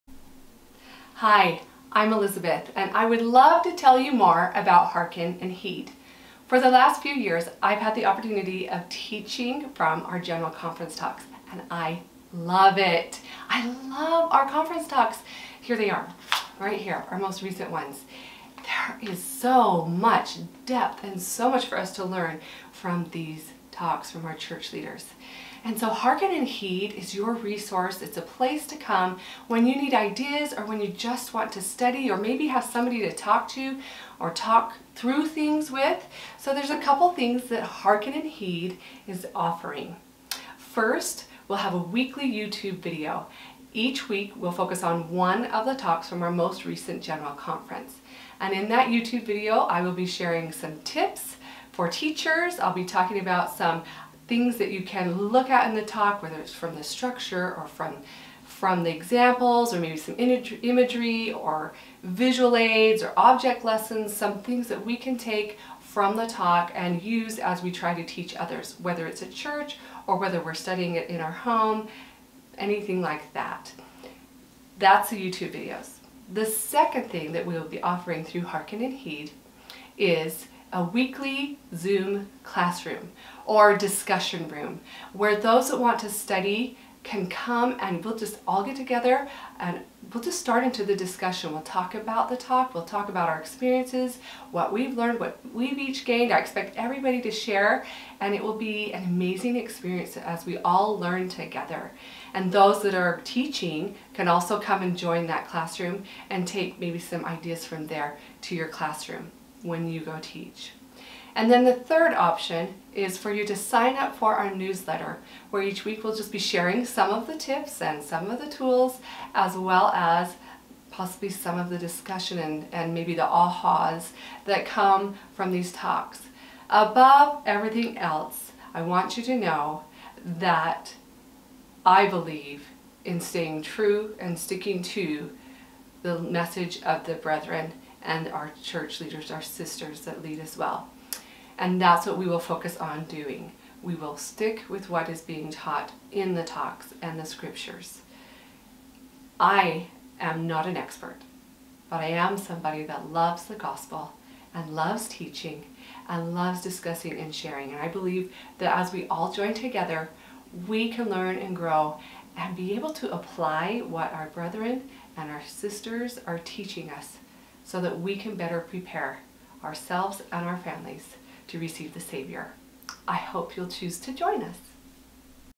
Hearken & Heed Introduction
Welcome and introduction to Hearked & Heed - a resource for those who wish to study and teach from LDS General Conference talks in greater depth. This video explains the three resources that are available to those who wish to participate in the Hearken & Heed community. For faithful, believing members of the Church of Jesus Christ of Latter-day Saints.